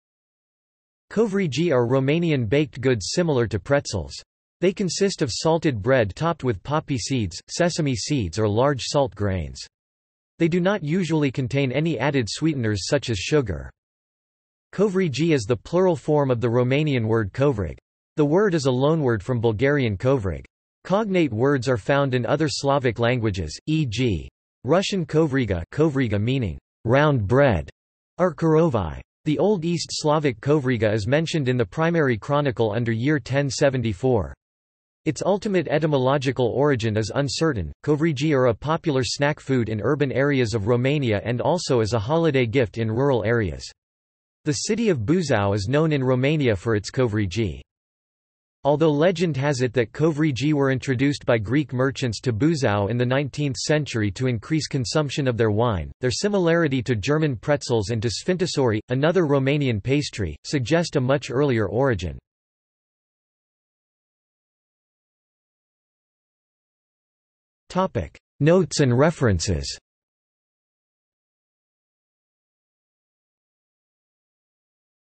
Covrigi | Wikipedia audio article
This is an audio version of the Wikipedia Article:
Covrigi

00:01:04 undefined

- Socrates

SUMMARY
Covrigi are Romanian baked goods similar to pretzels. They consist of salted bread topped with poppy seeds, sesame seeds or large salt grains. They do not usually contain any added sweeteners such as sugar.
Covrigi is the plural form of the Romanian word covrig. The word is a loanword from Bulgarian kovrig. Cognate words are found in other Slavic languages, e.g. Russian kovriga (коврига) meaning "round bread" or korovai. The Old East Slavic kovriga is mentioned in the Primary Chronicle under year 1074. Its ultimate etymological origin is uncertain.Covrigi are a popular snack food in urban areas of Romania and also as a holiday gift in rural areas. The city of Buzău is known in Romania for its covrigi.
Although legend has it that covrigi were introduced by Greek merchants to Buzău in the 19th century to increase consumption of their wine, their similarity to German pretzels and to sfințișori, another Romanian pastry, suggest a much earlier origin.